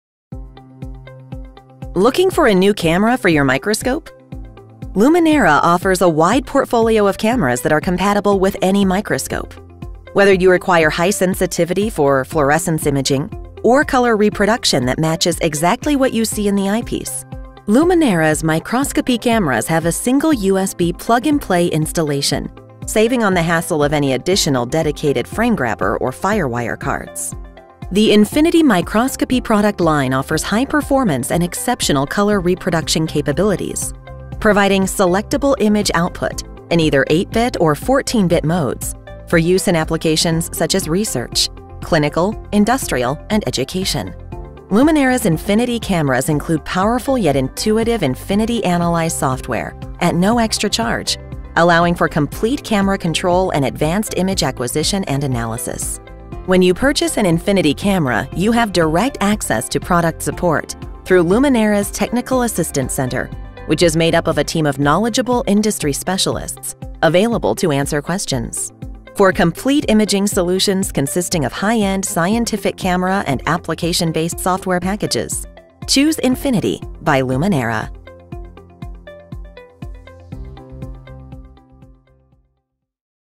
Need a Camera for Your Microscope? Choose Lumenera's INFINITY Microscopy Cameras
Lumenera's INFINITY cameras are compatible with any microscope, come with image analysis software, 4-year warranty, and exceptional customer support. Need help selecting a microscope camera?